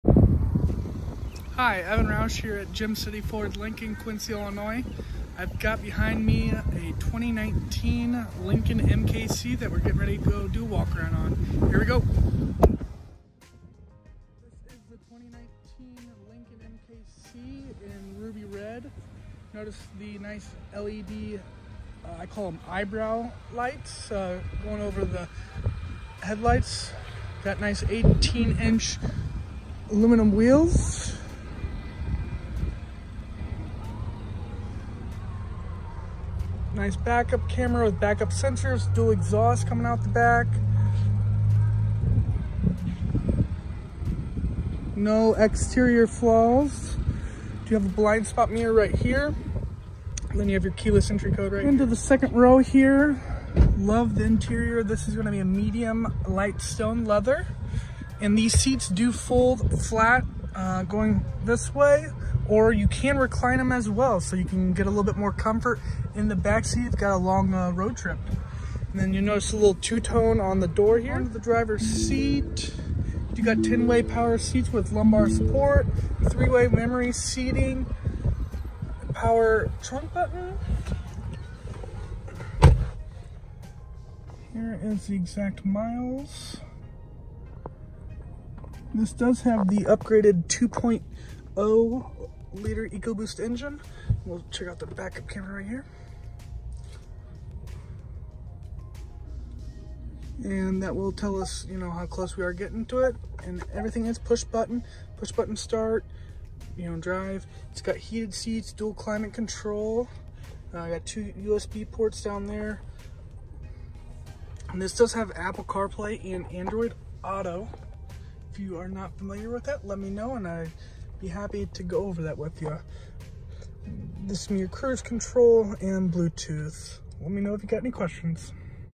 2019 Lincoln MKC AWD 2.0L
2019 Lincoln MKC AWD
LOCAL ONE OWNER TRADE ! ... 25 MPG ! ... 4WD ! ... HEATED FRONT SEATS with DRIVER MEMORY ! ... POWER LIFTGATE ! ... LINCOLN CONNECT 4G MODEM with WIFI CAPABILITY ! ... PUSH BUTTON START and SHIFT ! ... REAR CAMERA ! ... REMOTE START ! ... REVERSE SENSING SYSTEM ! ... SIRIUS and SYNC 3 ! ... ASE CERTIFIED INSPECTION ! ... FACTORY WARRANTY ! ...